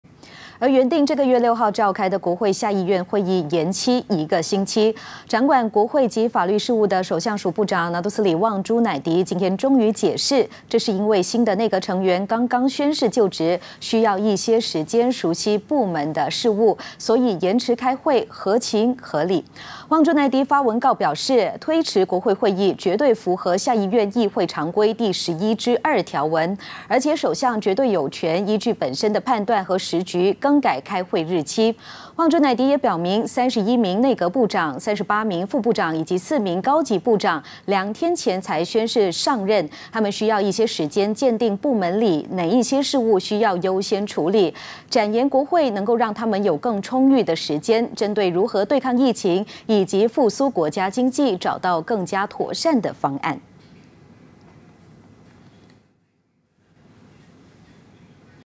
【新人需时熟悉部门事务 部长:推迟国会会议合法合理】
原定这个月6号召开的国会下议院会议延期一个星期。掌管国会及法律事务的首相署部长拿督斯里旺朱乃迪今天终于解释，这是因为新的内阁成员刚刚宣誓就职，需要一些时间熟悉部门事务，所以延迟开会合情合理。